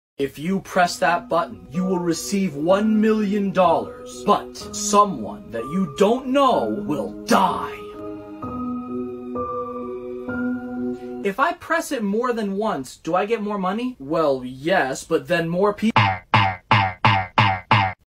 The Green Button
how many times would you press the green button?

In this short video, we're exploring the danger of pressing the red button. $1M or 1M lives?

credit: boygeniusepisodes